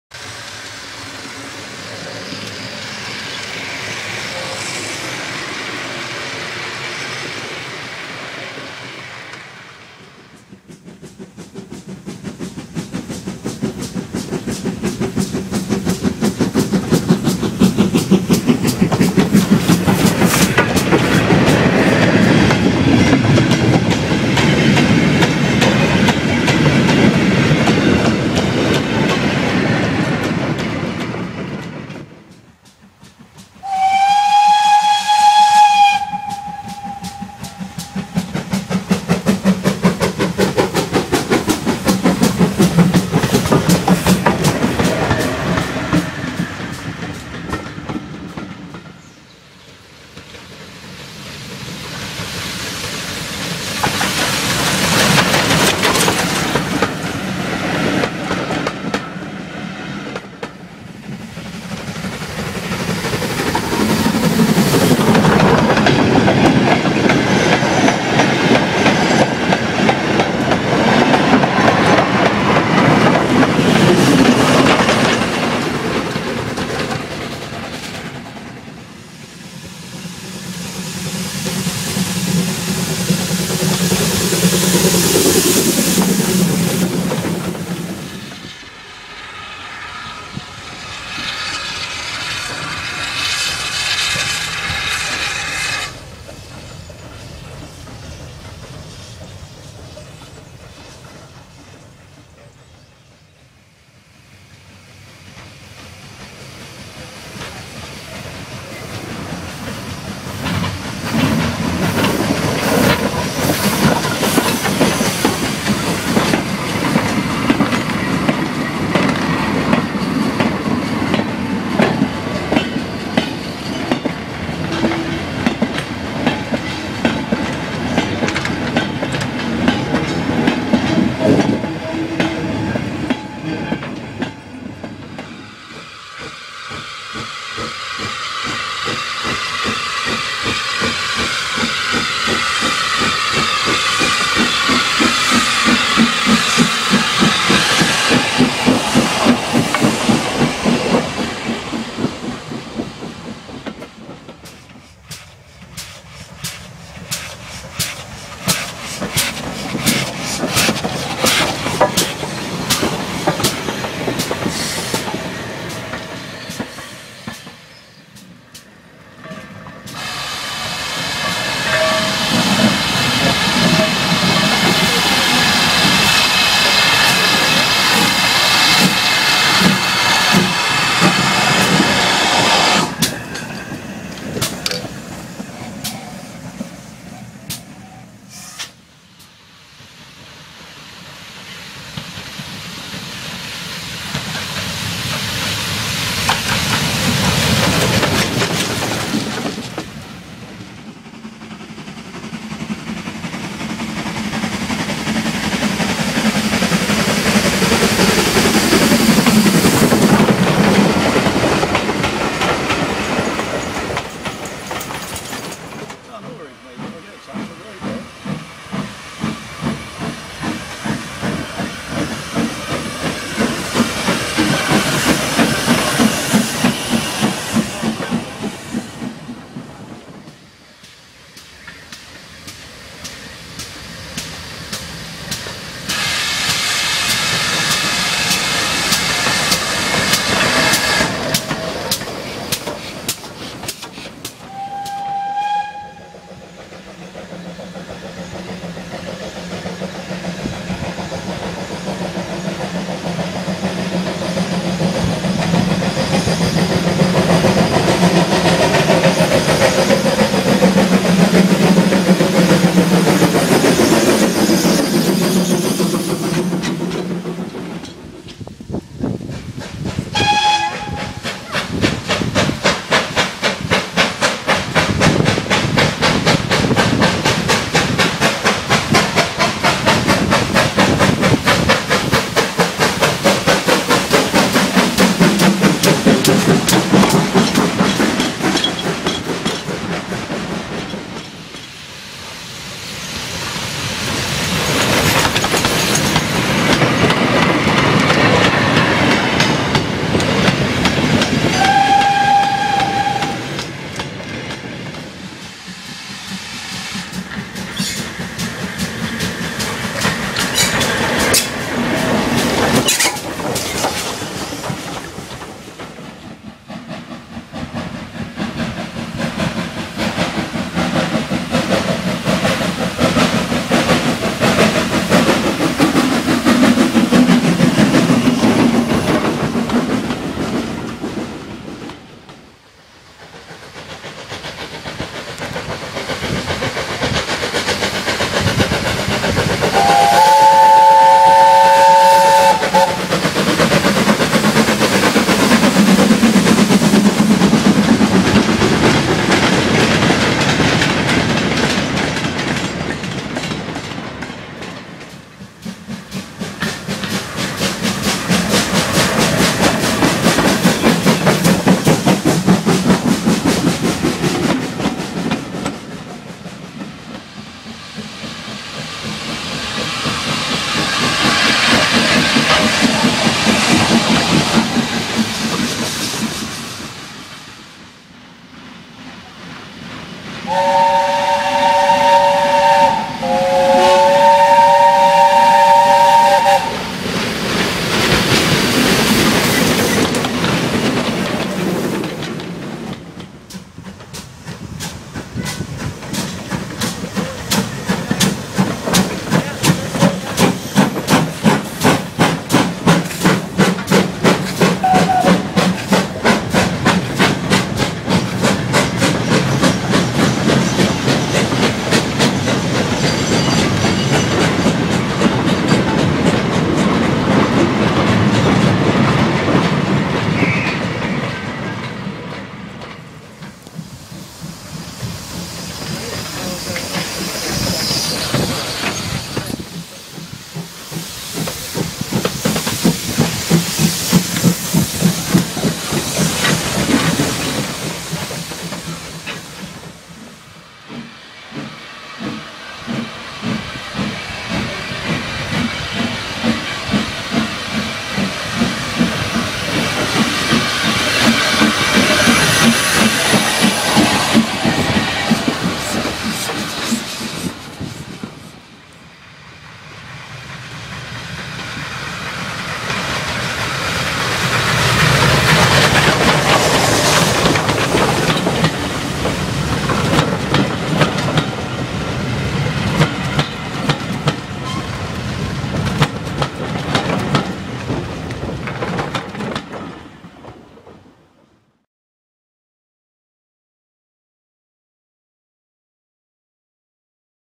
Avon Valley Railway End of Season Gala 2013
Avon Valley Railway End of Season Gala, on the 19th October 2013.
Including freight, non-stop runs and even a triple header.
Feat:
GWR Manor Class, No. 7822, 'Foxcote Manor'
•RSH 0-6-0T, No. 7151
•Ferrum 0-6-0T, No. 4015, 'Karel'
•Austerity 0-6-0ST, No. 15, 'Earl David'
Thanks to, SDJR7F88, MrAbbeyMead, itfcsam and 6024mad for meeting up on the day